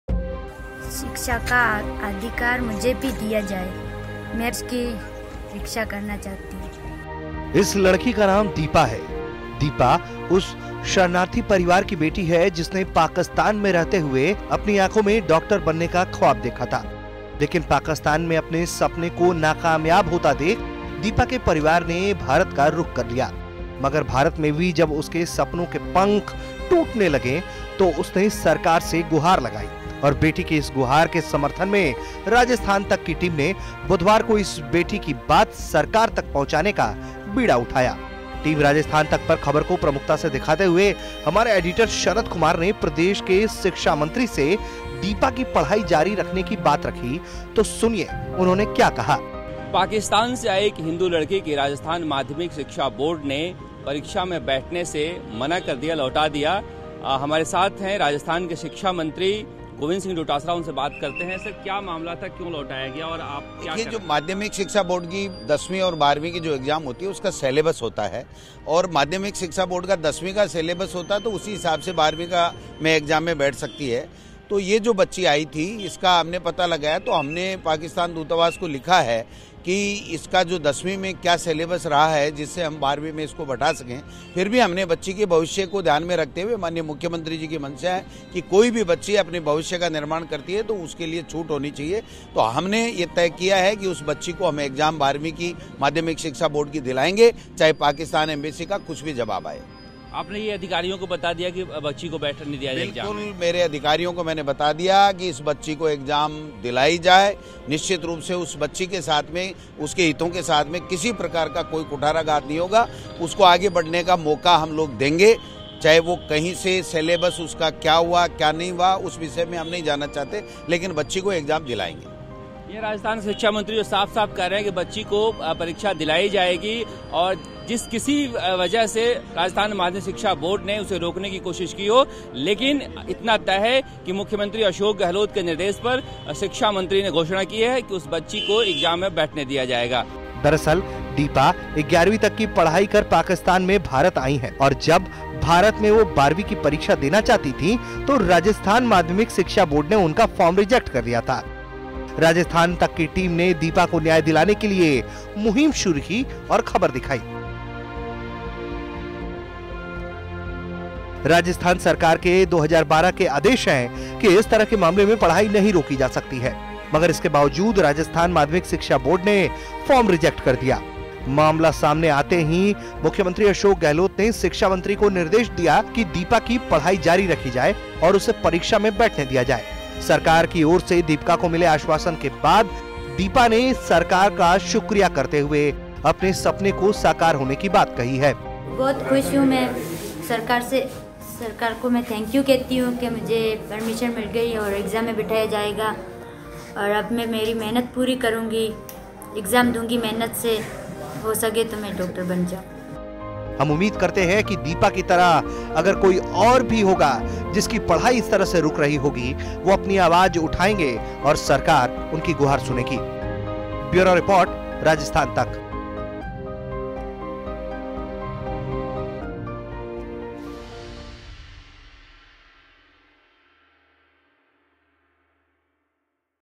सीएम ASHOK GEHLOT ने सुनी पाकिस्तानी हिन्दू बेटी की गुहार !
जोधपुर में पाक विस्थापित हिन्दू दीपा का सपना पूरा करेगी प्रदेश की गहलोत सरकार !